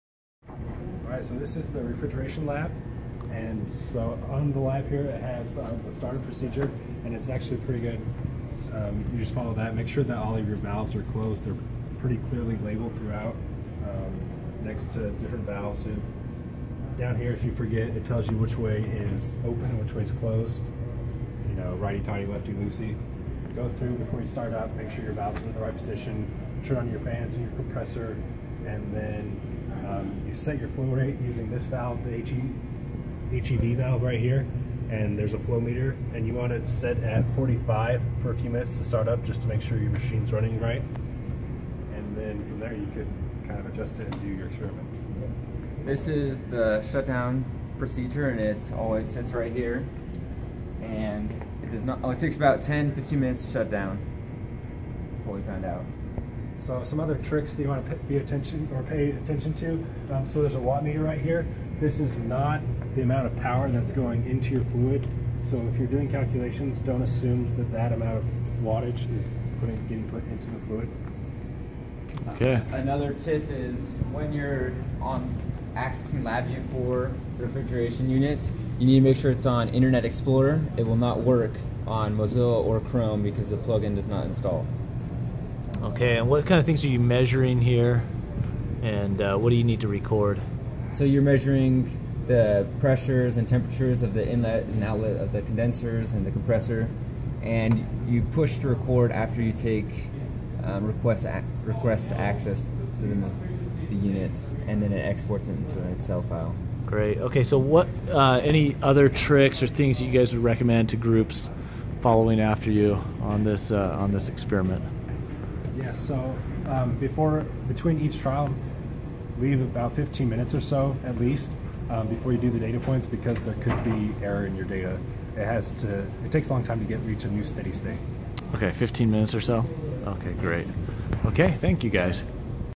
How to Run a Refrigeration System for Engineers
The unit operations lab includes a refrigeration experiment with flows, temperatures, and pressures measured to investigate the Coefficient of Performance (COP).